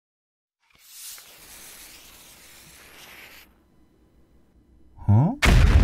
[SFM/TTTE] Oh God, Henry Has Dynamite.
oh great heavens! it seems that Henry the Green Engine has gotten his hands on a big stick of kaboom juice. whatever hijinks will ensue? probably an explosion. like its dynamite what do u expect?